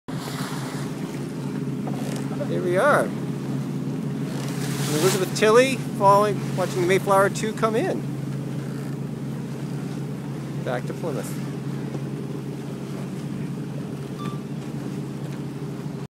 The Mayflower II returns to Plymouth.
Taken on 7 August 2013 from the deck of the 1628 Shallop Elizabeth Tilley, which is owned by The Pilgrim John Howland Society.